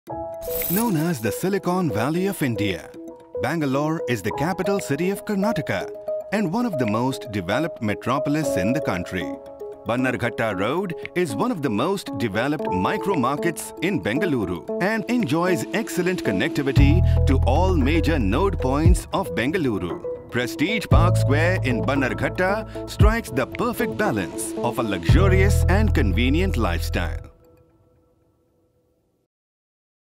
Short Video | 30 sec | Connectivity
A 30 secs short video of Prestige Park Square's connectivity to Bannerghatta Road and other major nodes in Bangalore.